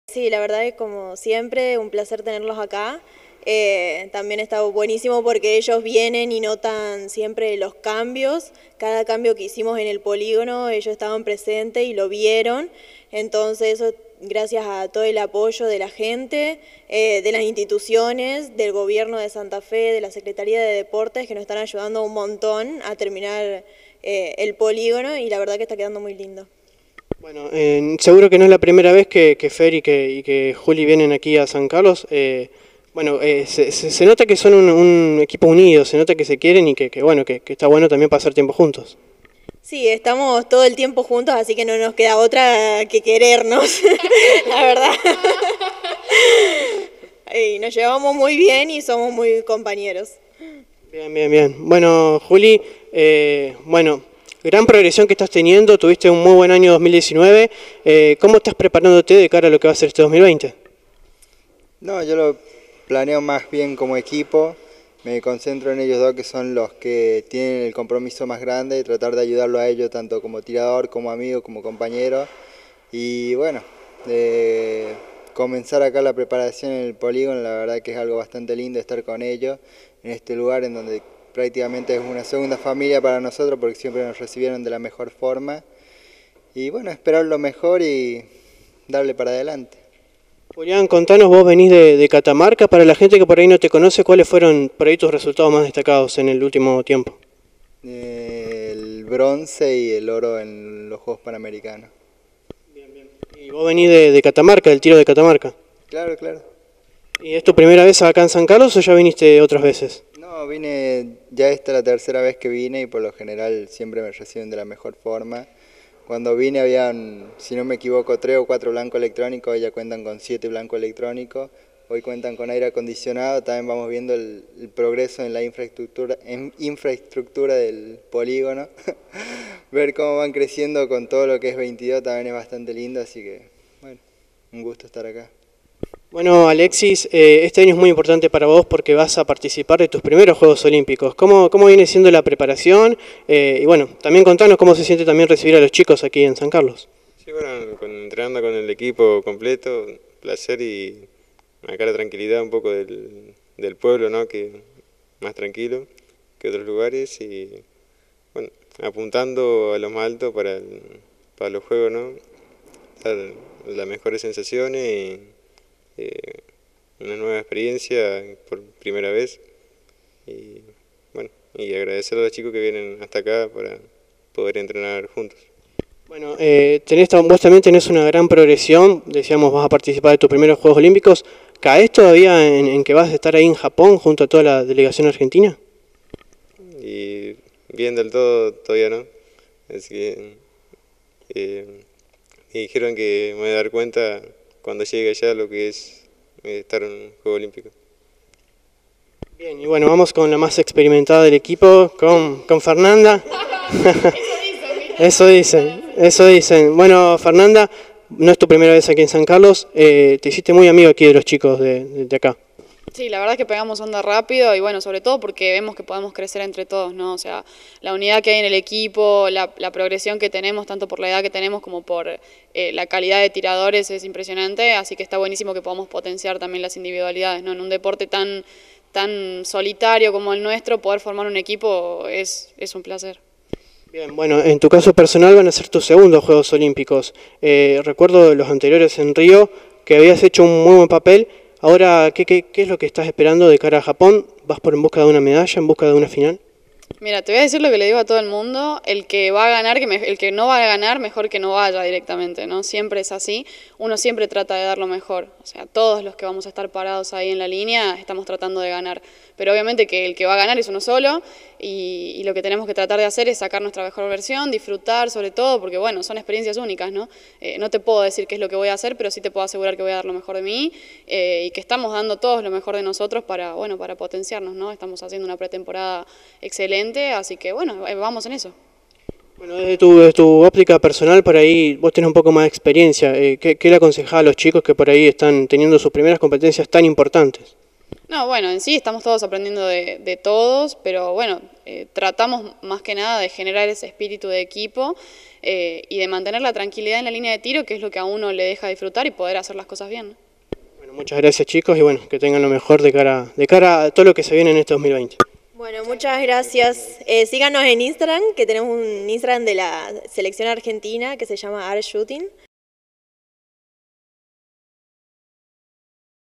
Alliana Volkart – Julián Gutiérrez – Alexis Eberhardt – María Fernanda Russo – Tiro
La Selección Nacional pasó por San Carlos